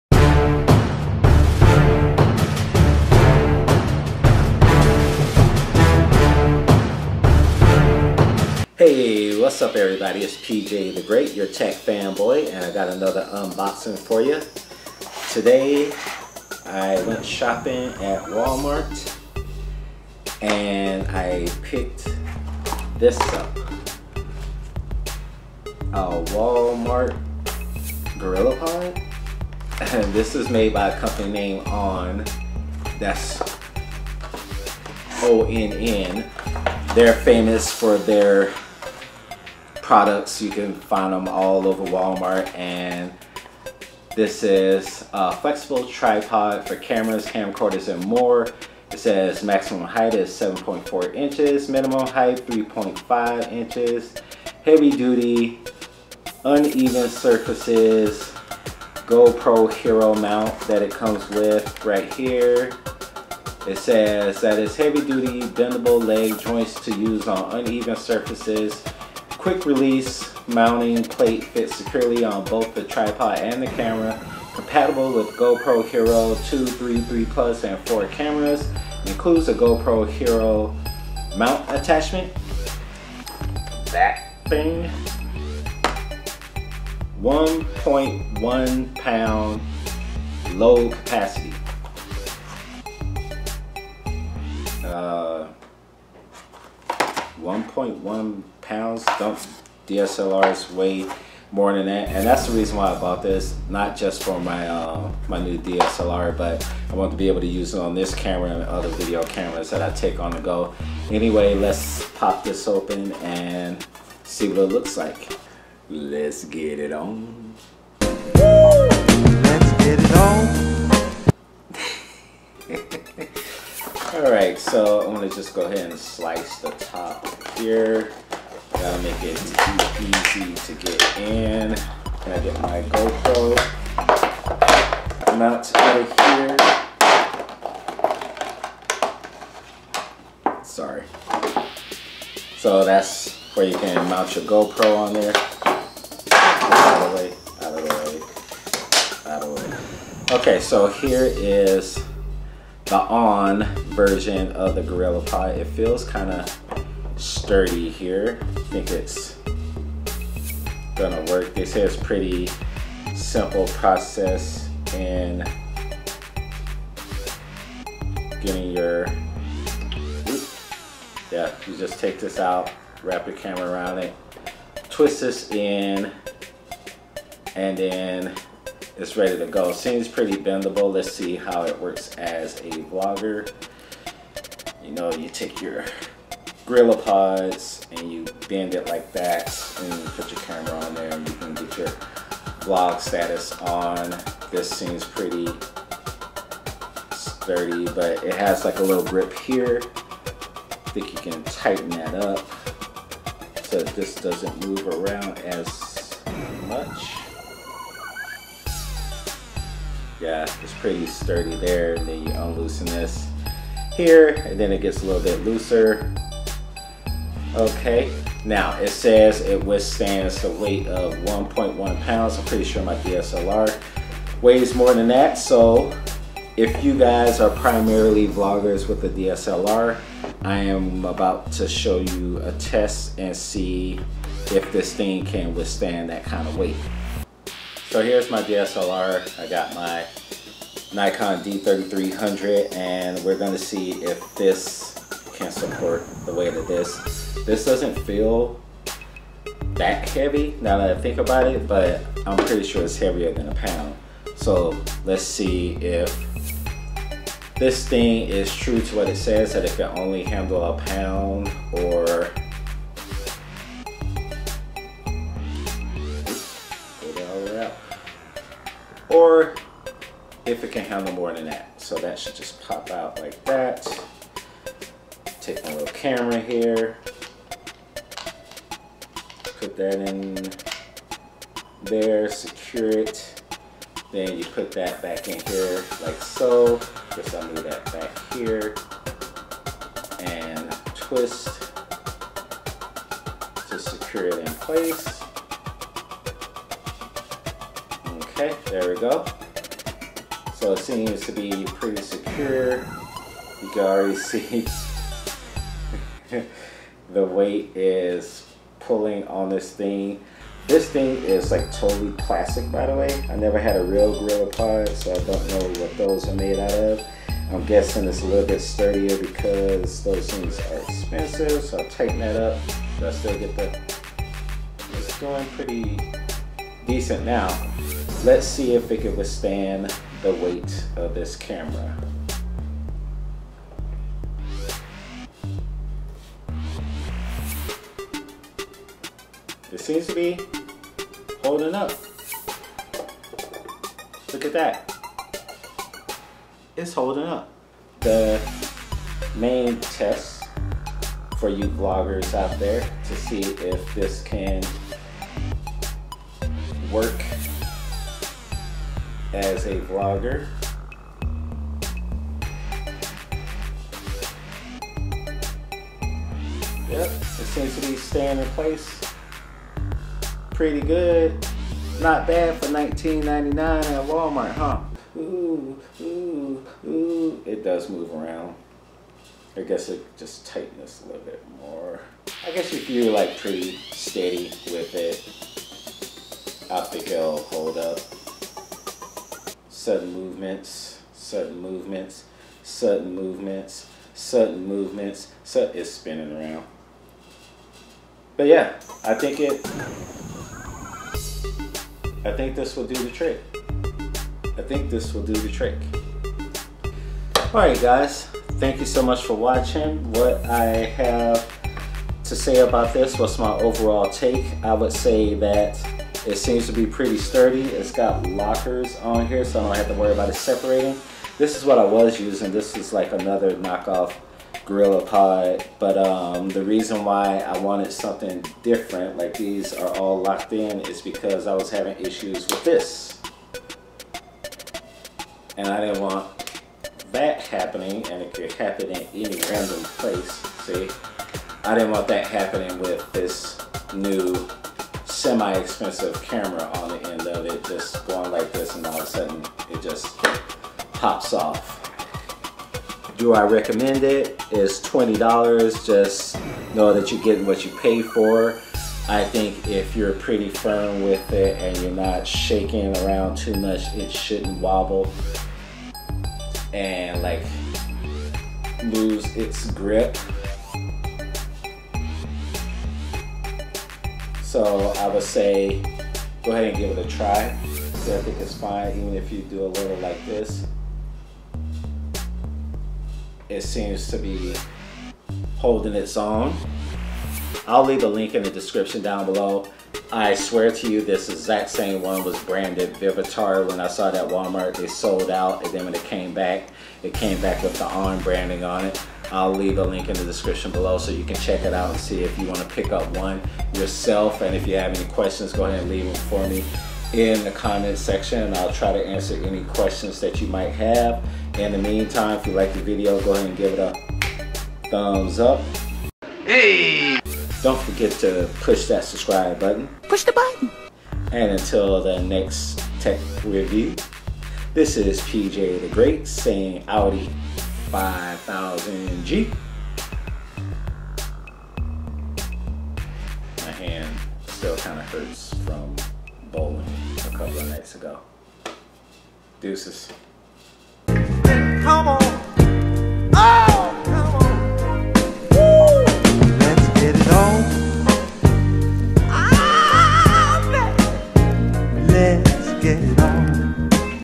Onn Flexible Tripod - The Walmart Gorilla Pod?
Hey everybody! Today I share my new purchase of the Onn Flexible Tripod. This is an affordable alternative to the Gorilla Pod at only $19.99 at Walmart.

I test it out to see how durable it is and if it can withstand the weight of a traditional DSLR camera. It holds its own, but I will have to do some field testing to see how it does.

If you want this for just a point-and-shoot camera or a lightweight camcorder, this will do the business for cheap. I'm not quit sure how it will withstand the weight of a DSLR while making sudden movements, so just be aware. I plan to try it on those random occasions I use my Nikon D3300 as a vlogger to see how it holds up.

If you're interested in picking one up, check out the link here at Best Buy. It's labeled as a Targus, but as mentioned in the video I've seen it branded as Vivitar as well. I checked the Walmart website and the Onn version isn't listed, but I found it in my local store here in Las Vegas.

What's in my repertoire?

Canon Vixia HF R700 w/ Vivitar Zoom Lense
Sony HDR-CX240
Nikon D3300
Click+ Action Camera